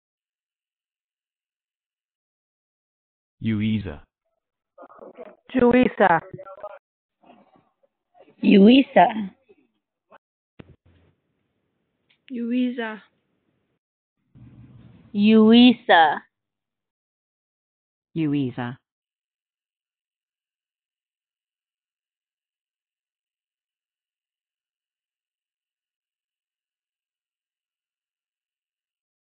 How to Pronounce Yuiza
This video shows you how to pronounce Yuiza. Record your own pronunciation, view the origin, meaning, and history of the name Yuiza